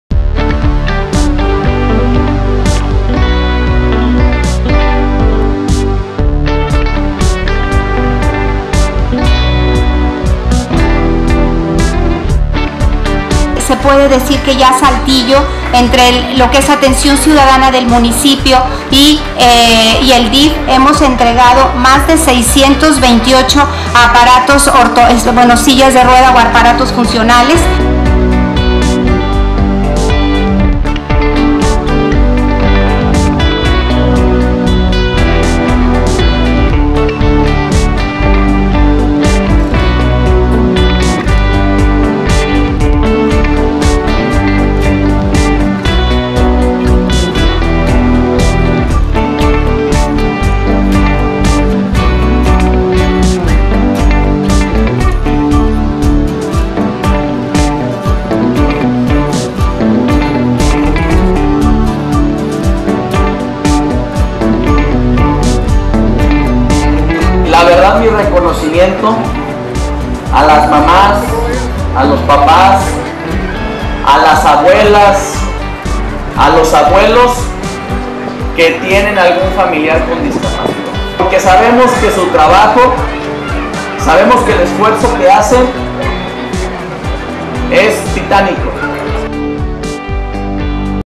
Entrega Manolo Jiménez sillas de ruedas y aparatos ortopédicos
El alcalde de Saltillo, Manolo Jiménez Salinas, encabezó la entrega de sillas de ruedas y aparatos ortopédicos a beneficiarios del DIF Saltillo. (JOSÉ TORRES)